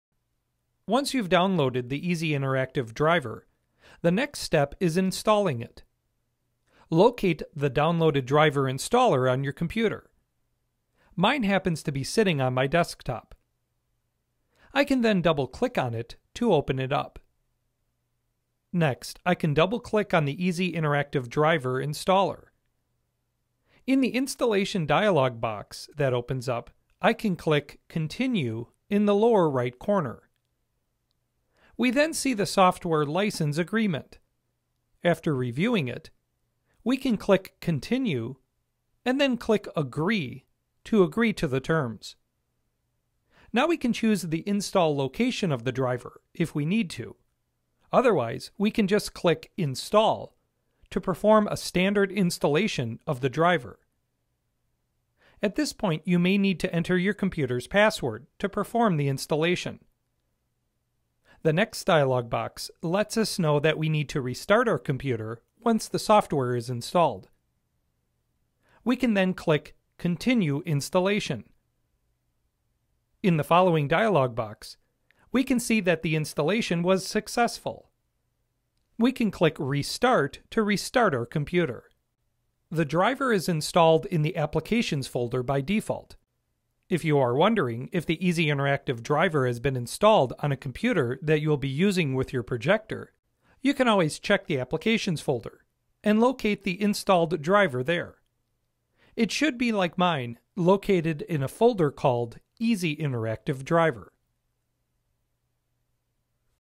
Epson BrightLink Projectors | How to Install Easy Interactive Driver on a Mac
Learn how to install the Easy Interactive Driver required for interactivity when BrightLink 436Wi, 475Wi, 480i, or 485Wi interactive projectors are connected to a Macintosh.